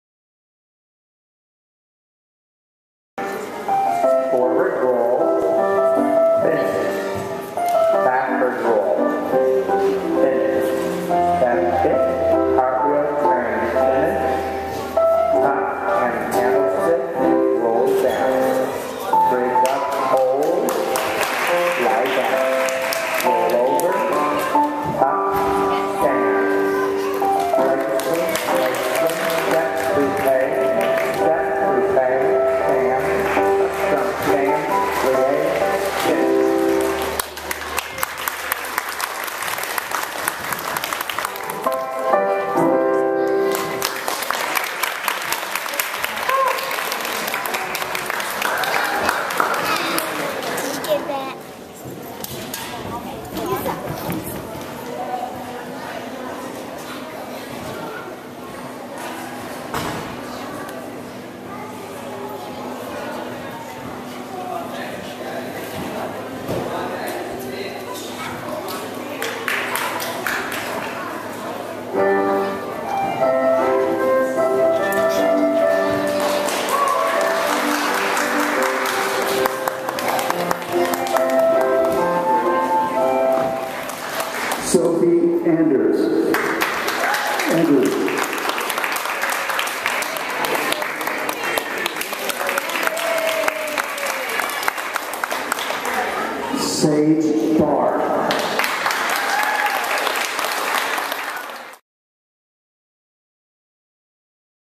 Gymtowne Snowflake Festival Feb 2011
Sophie in the Snowflake Festival at Gymtowne. February 5th 2011.